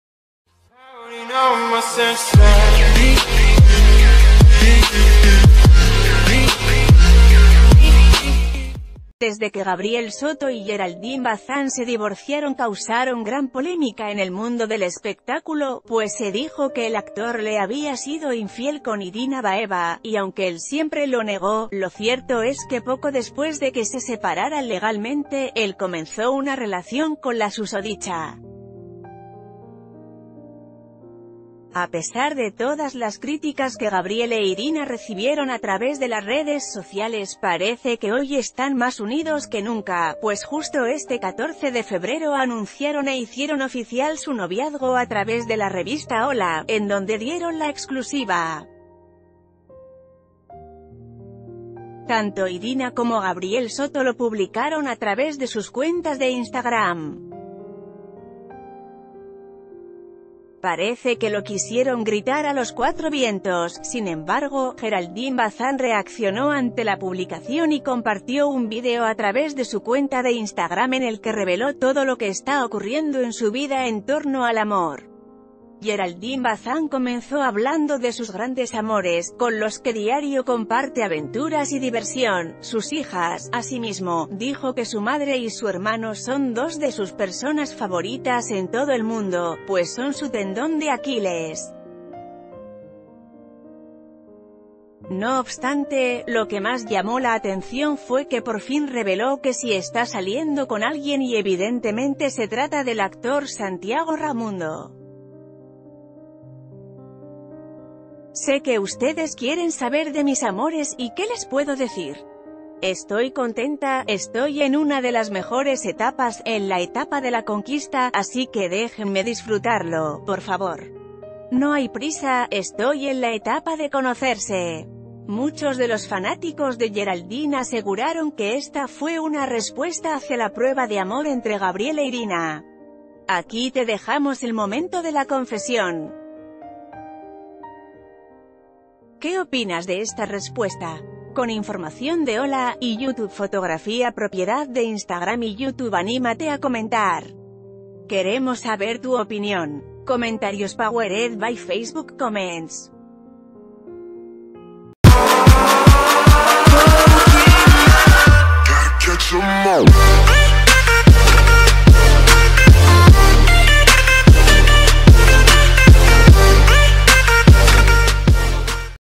Geraldine Bazán reacciona ante prueba de amor de Gabriel Soto e Irina Baeva
Desde que Gabriel Soto y Geraldine Bazán se divorciaron causaron gran polémica en el mundo del espectáculo, pues se dijo que el actor le había sido infiel con Irina Baeva, y aunque él siempre lo negó, lo cierto es que poco después de que se separaran legalmente, él comenzó una relación con la susodi...